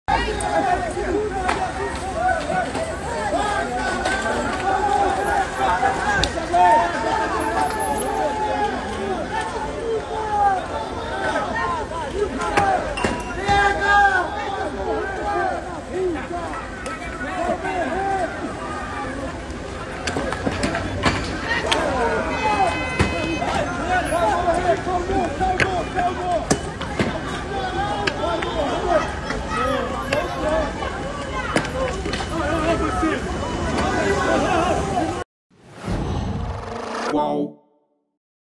Vídeo mostra PMs sendo atingidos por pedras na Cracolândia
Rio Branco, em São Paulo. Segundo a PM, o agente foi atacado durante uma abordagem e acabou ferido. O policial foi levado para o Hospital das Clínicas e duas pessoas foram detidas.